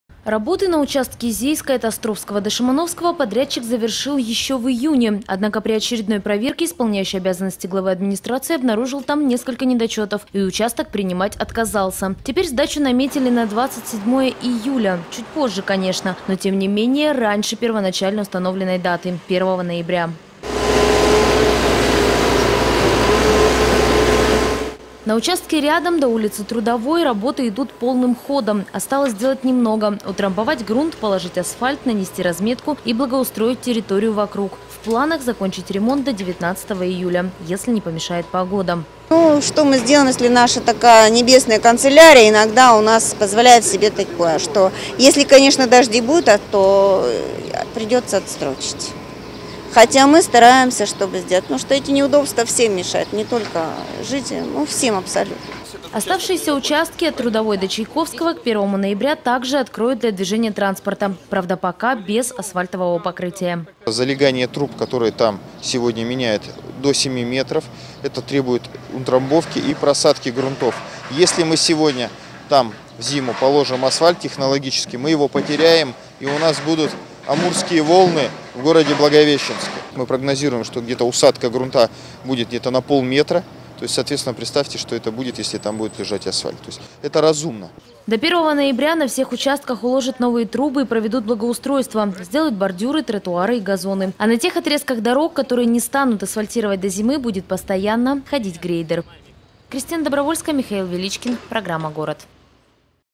Реконструкция канализационного коллектора в Благовещенске завершится в этом году
Подрядчики пока укладываются и даже опережают установленный администрацией срок - до 1 ноября. Как идут работы на одном из участков, проверил исполняющий обязанности главы администрации Александр Козлов.